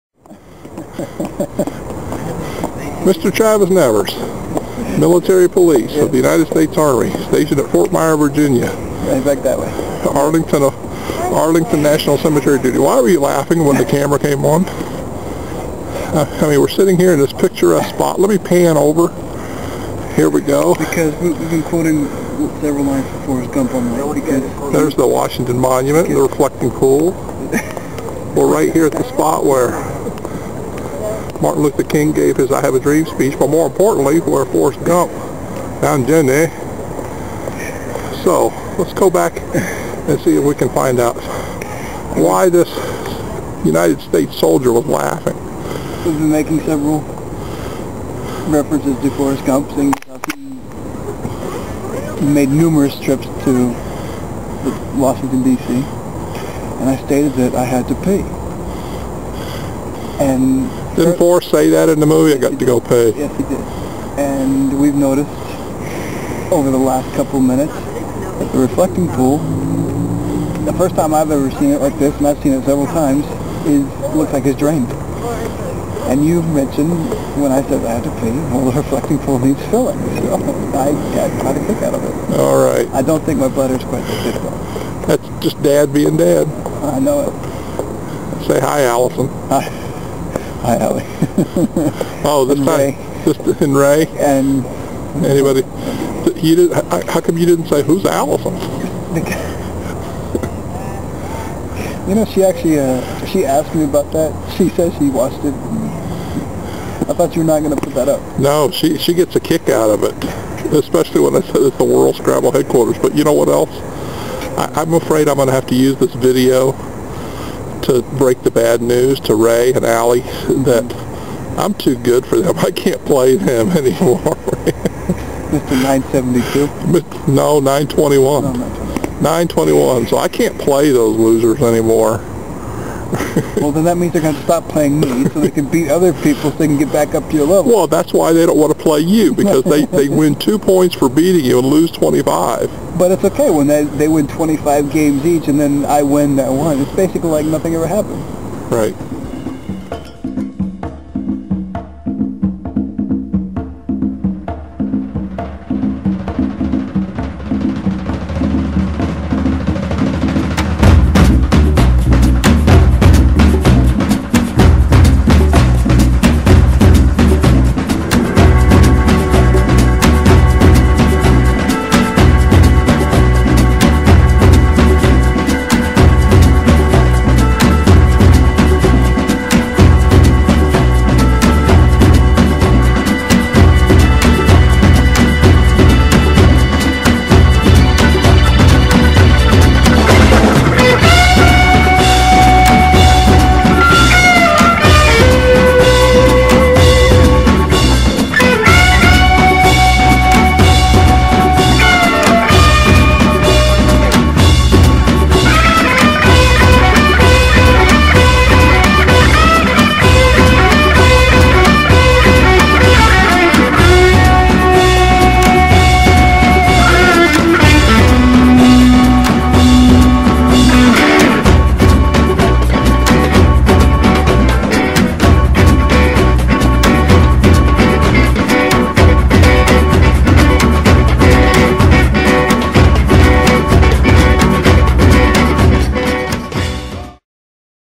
TRAVIS NEVERS 2010 Having some laughs with Brent at The Lincoln Memorial
Brent Nevers and son Travis share some laughs and good memories while sitting on the steps of the Lincoln Memorial in Washington Dc during the winter of 2010. !